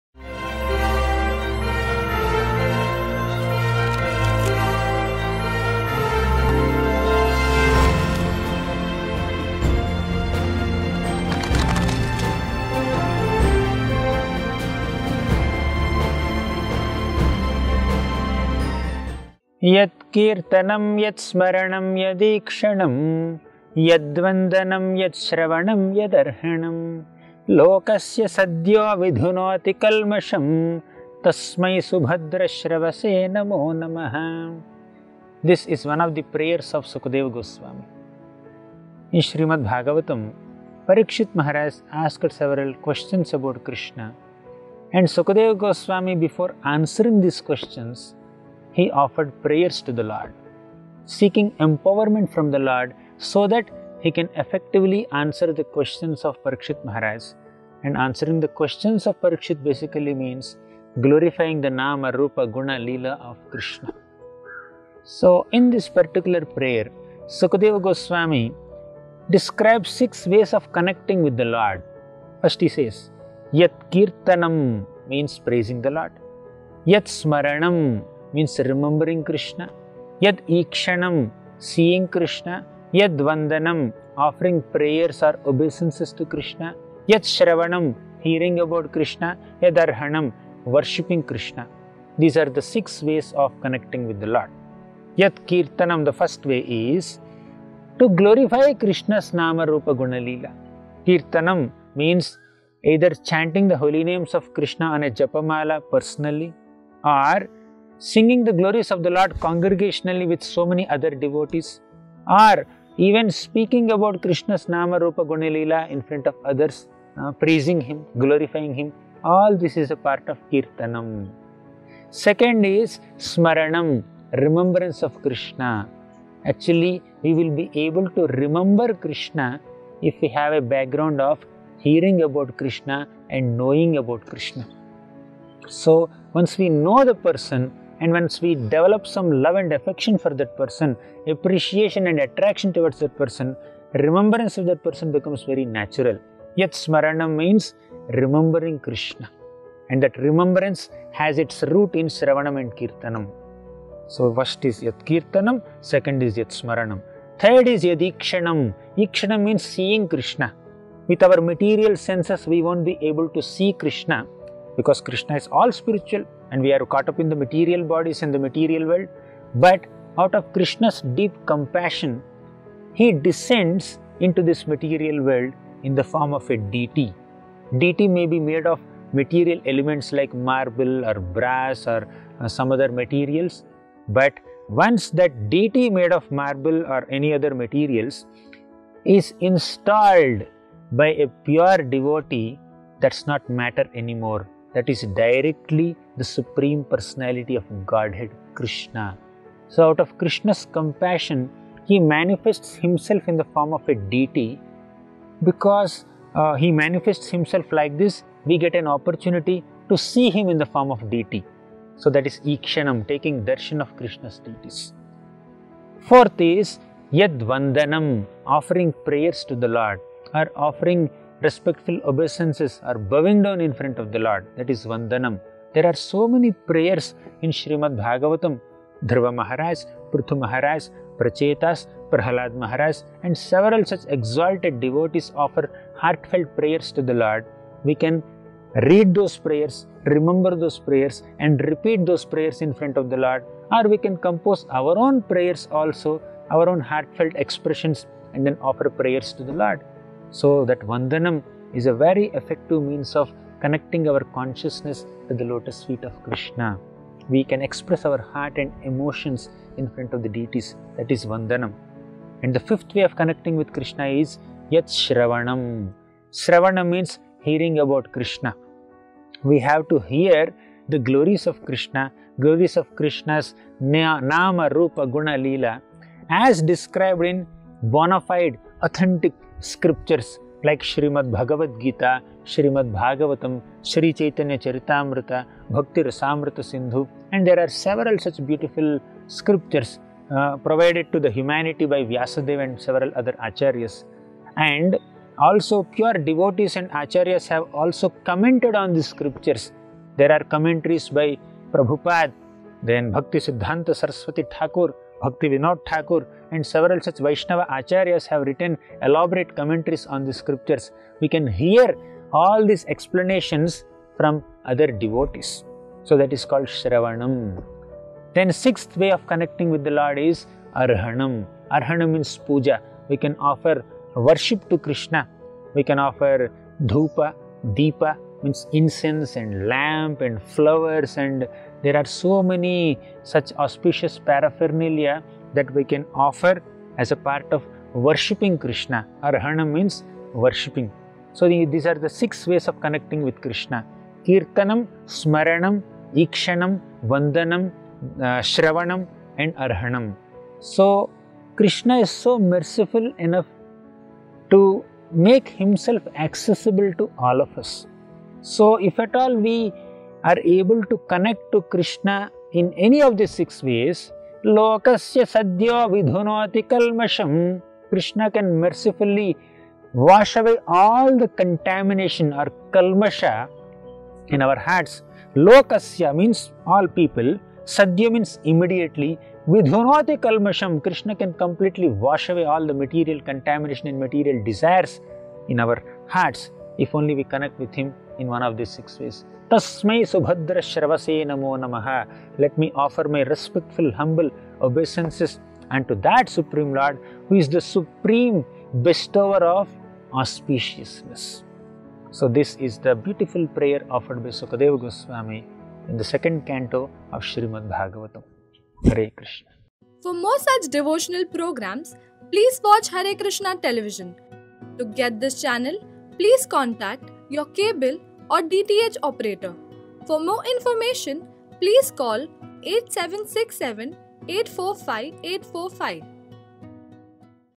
6 Way Of Connecting To God | Nectar Verses SB 2.4.15 | Gauranga Darshan Das
In todays Spiritual Enlightenment video Gauranga Darshan Das explains 6 Way Of Connecting To God from SB 2.4.15 . Watch video till end to know more

ŚB 2.4.15
यत्कीर्तनं यत्स्मरणं यदीक्षणं
यद्वन्दनं यच्छ्रवणं यदर्हणम् ।
लोकस्य सद्यो विधुनोति कल्मषं
तस्मै सुभद्रश्रवसे नमो नम: ॥ १५ ॥

Translation
Let me offer my respectful obeisance's unto the all-auspicious Lord Śrī Kṛṣṇa, about whom glorification, remembrances, audience, prayers, hearing and worship can at once cleanse the effects of all sins of the performer.

Watch 24x7 free to air satellite television channel "Hare Krsna TV" on your Television set/ Mobile/ Tablet / Laptop /PC.